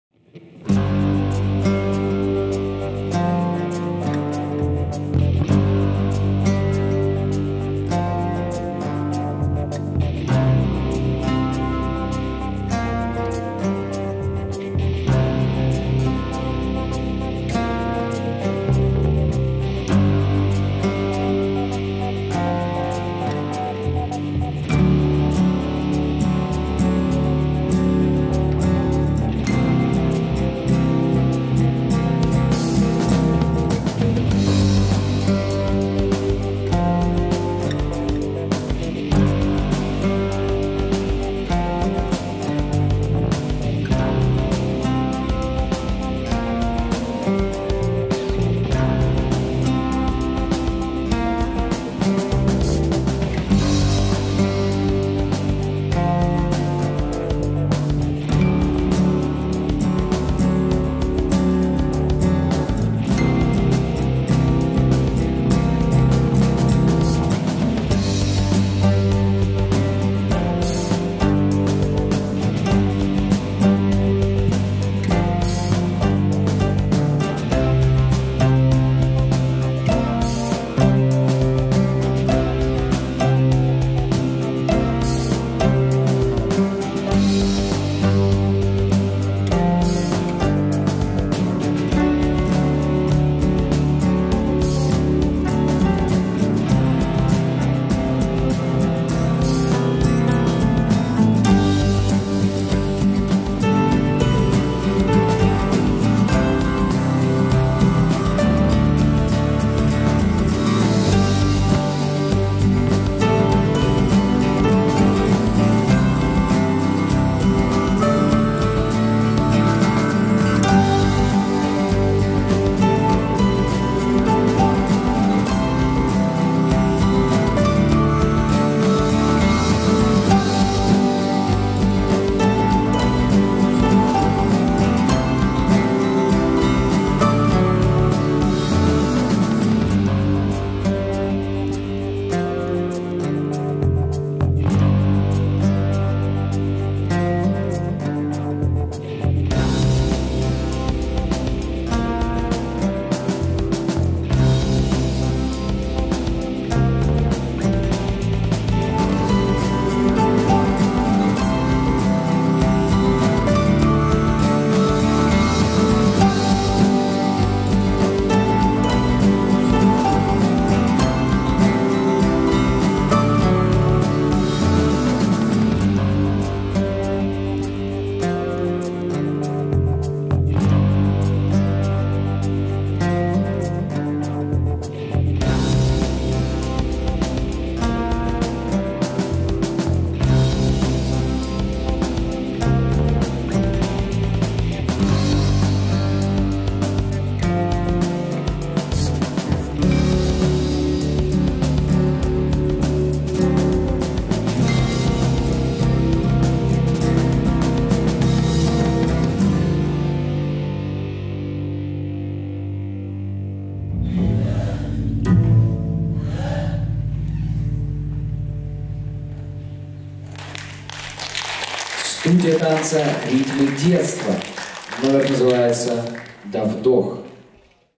"Довдох". Современный танец.
Студия танца "Ритмы Детства"
I Отборочный тур Международного фестиваля-конкурса хореографического искусства "Геометрия танца"